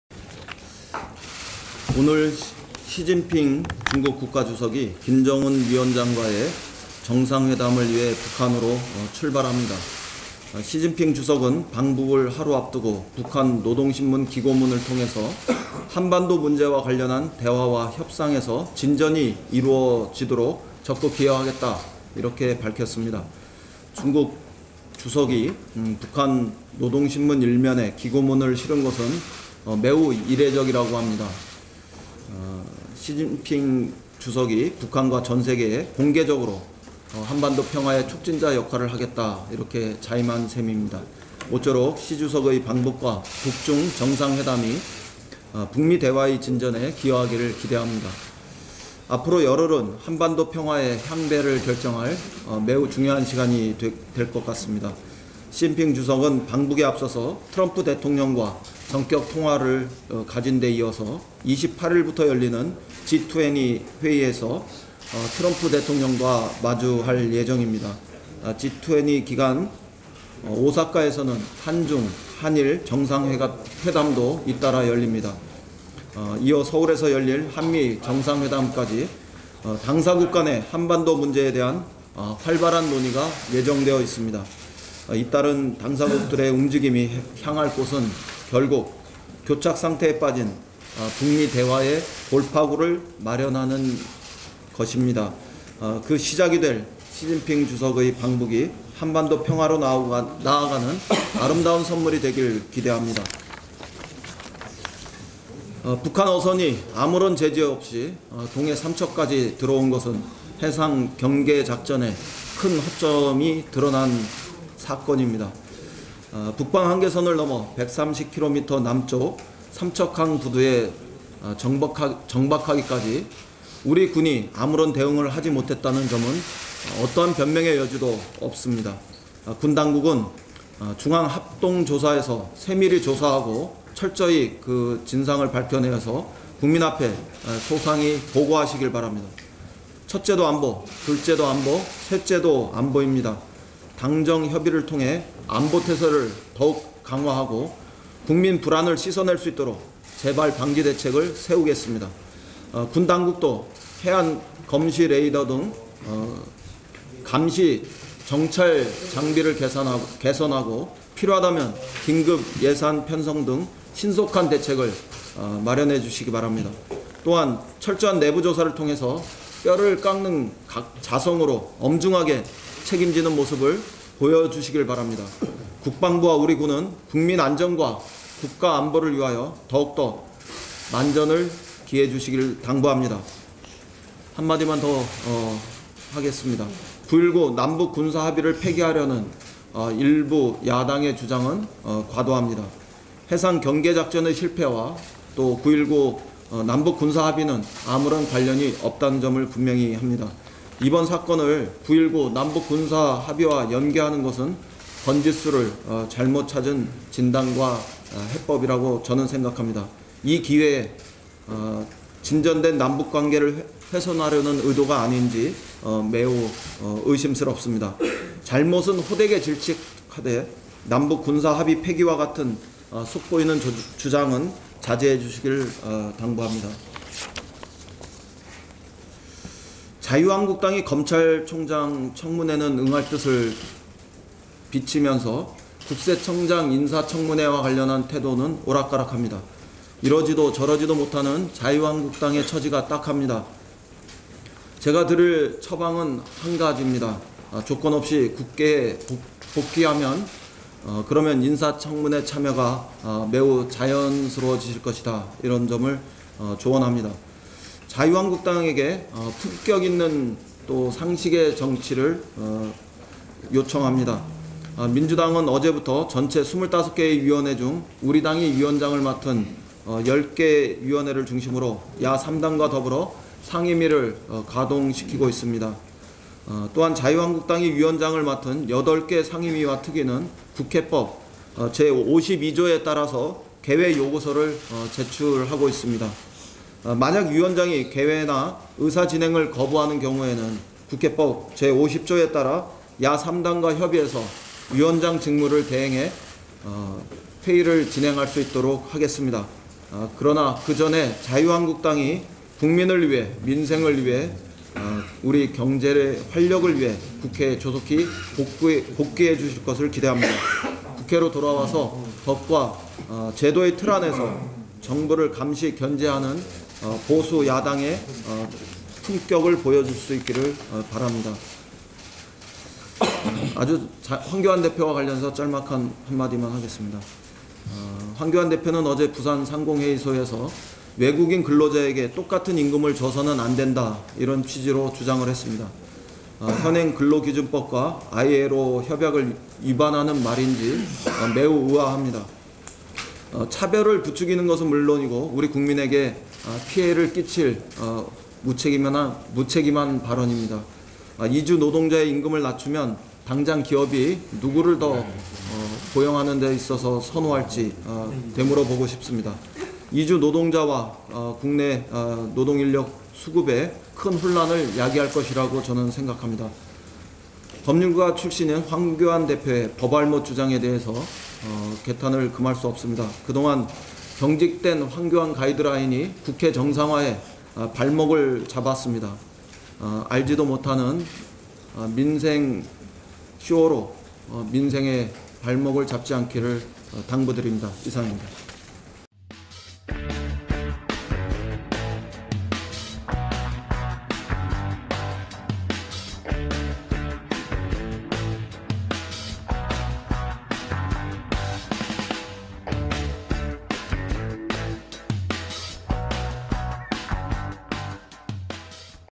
민주당 이인영 "한국당, 이러지도 저러지도 못하는 처지 딱해…국회 복귀하길"
6월 20일 진행된 더불어민주당 정책조정회의에서 이인영 원내대표는 자유한국당을 언급했다.